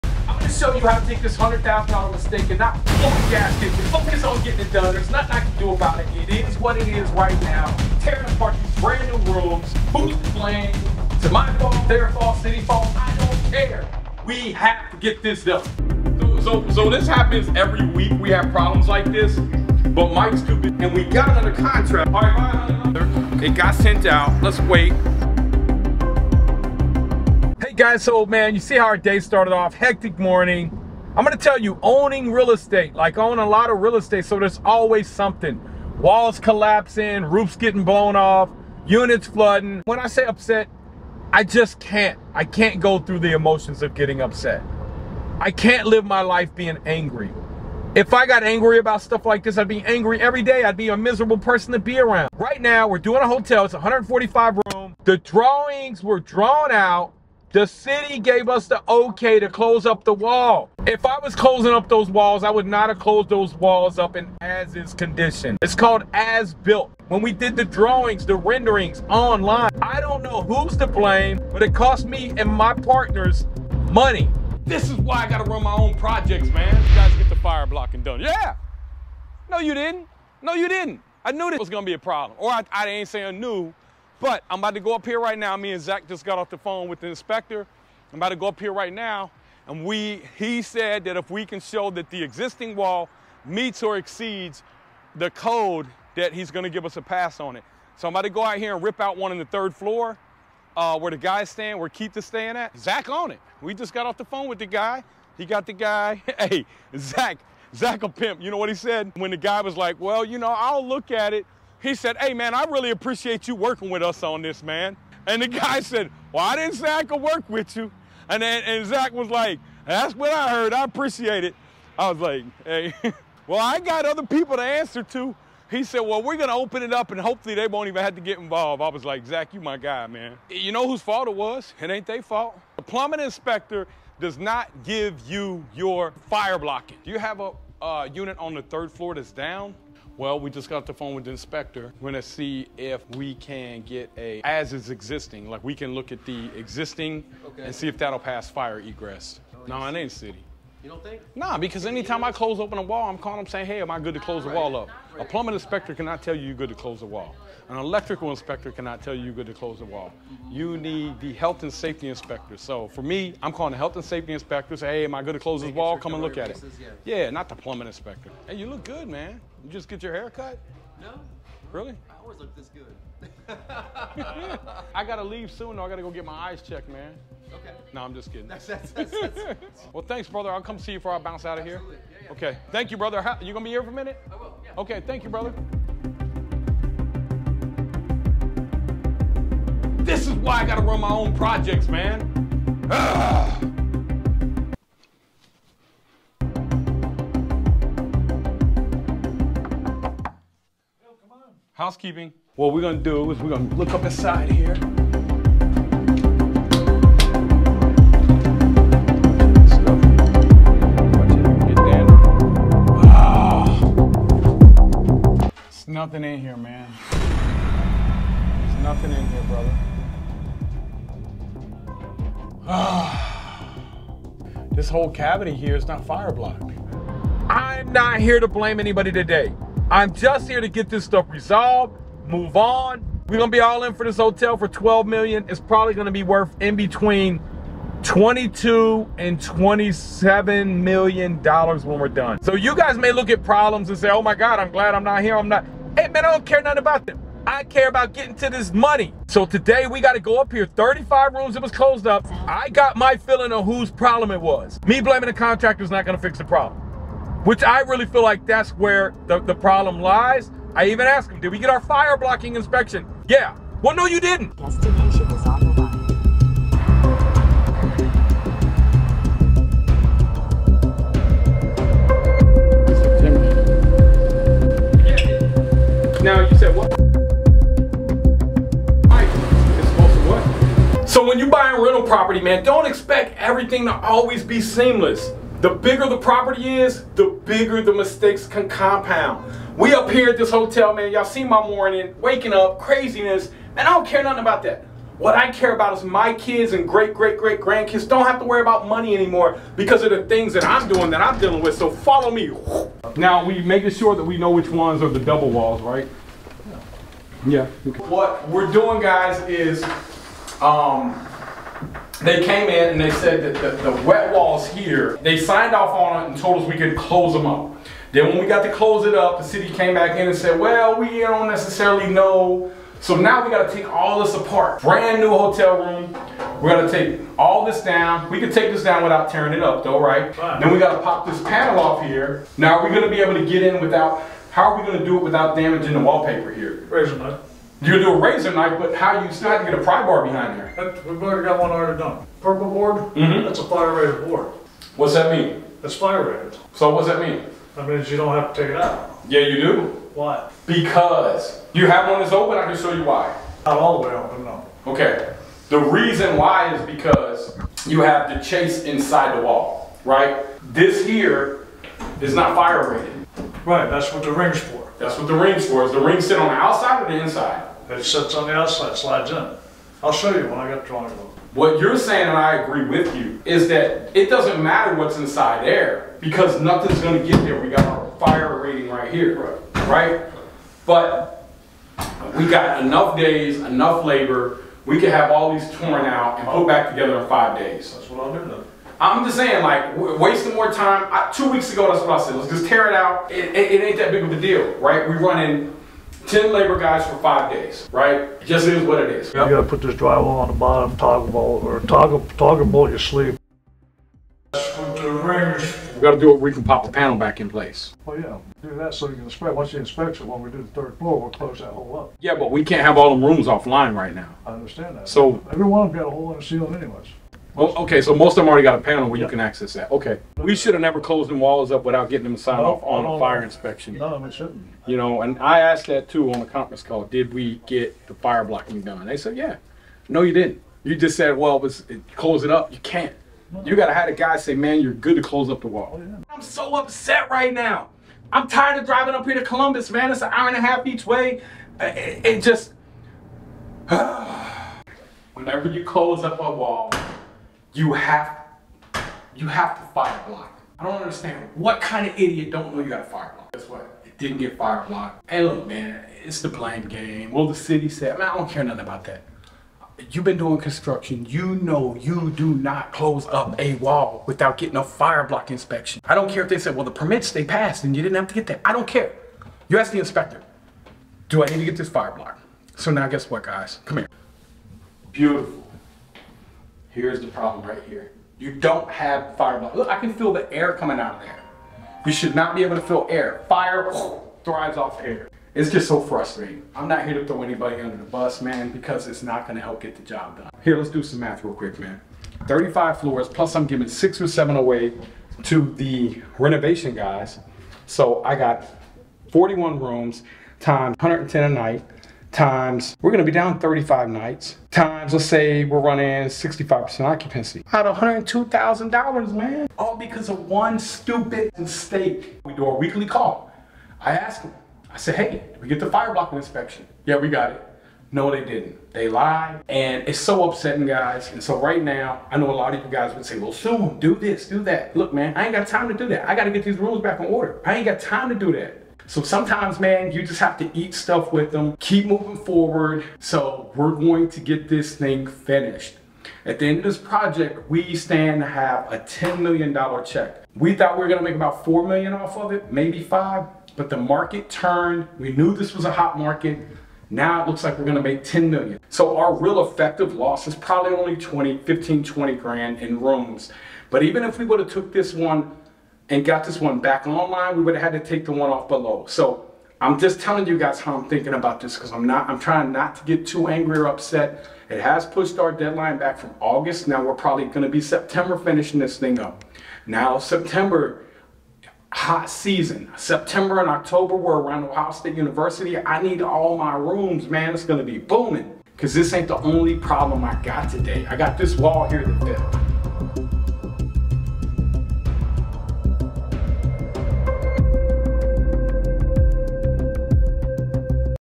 Losing $100,000 On One Mistake!
I'm going to show you how to take this $100,000 mistake and not blow a gasket, focus on getting it done. There is nothing I can do about it anymore except fix it. At the end of the video see how I determine this is a $100,000 mistake minimum!!!